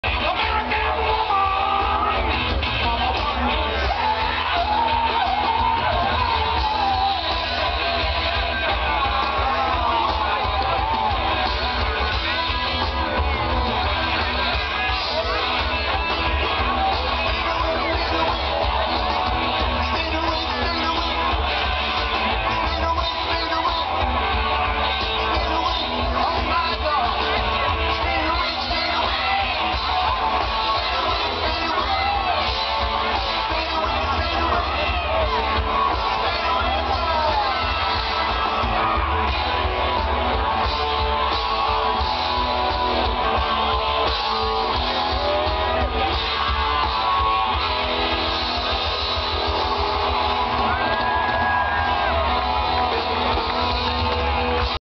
Bachman Turner Overdrive at Summerfest
We attended the concert portion of the 3-day Summerfest in Charlottetown, PEI. Matt Mayes and BTO opened for Great Big Sea.